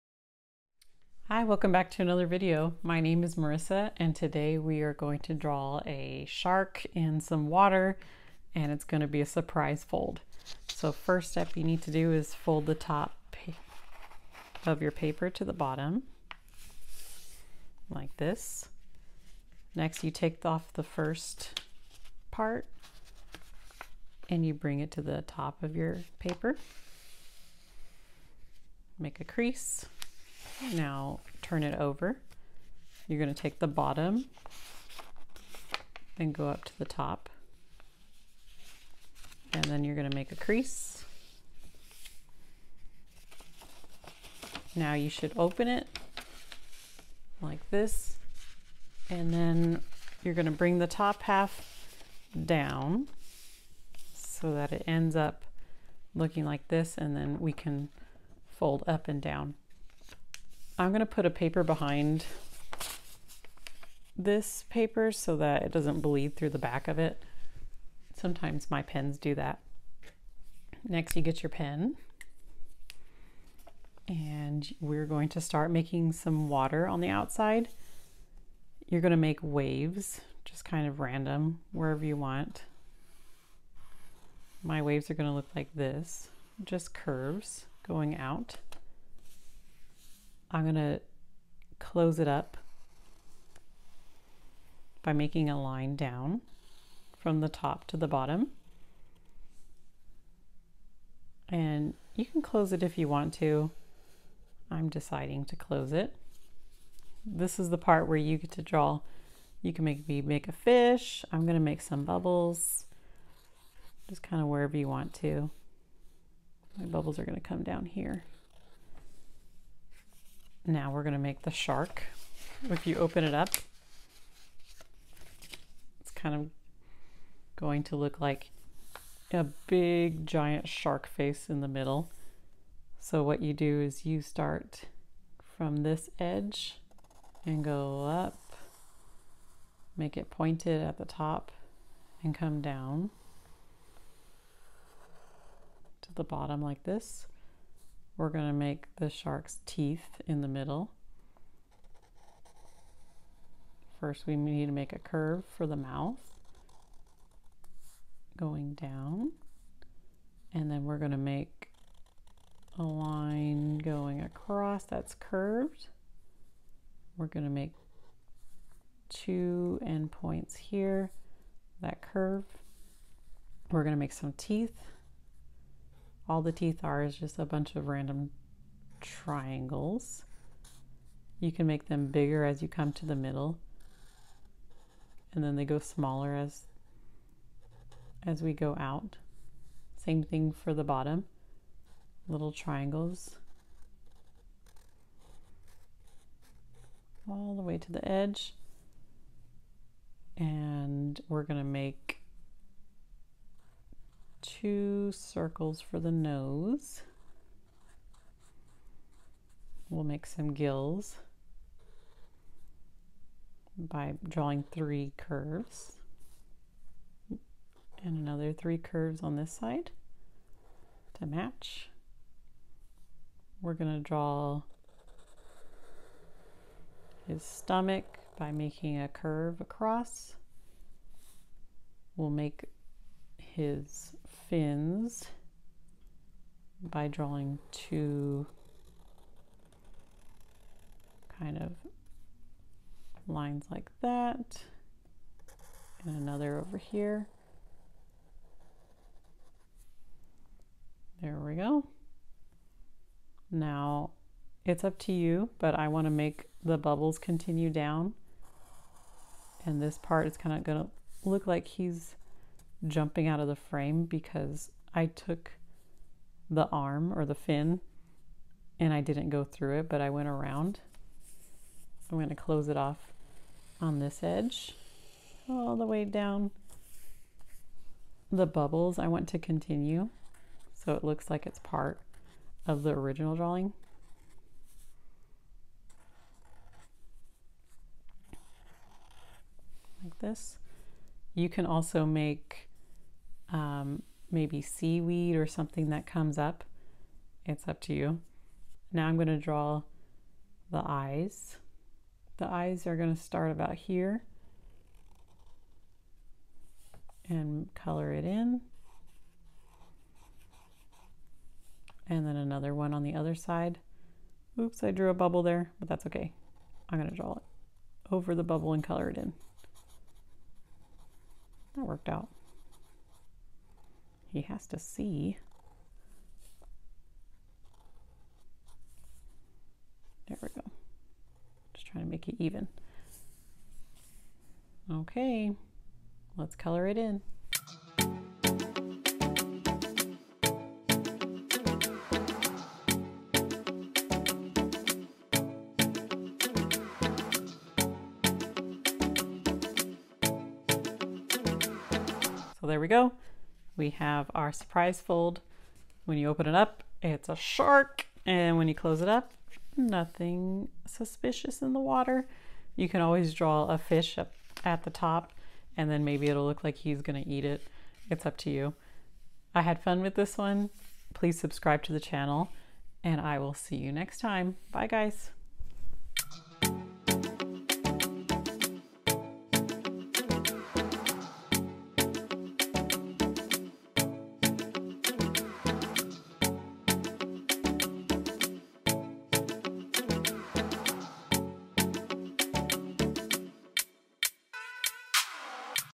How to Draw a Shark! - Surprise Fold!
Join me in drawing a SHARK! This is a fun surprise fold drawing. You can even draw more sharks or different ocean animals to put in the water. Have fun with this art for kids drawing! It is a great distance learning art activity for kids as well for zoom, teachers, google classroom.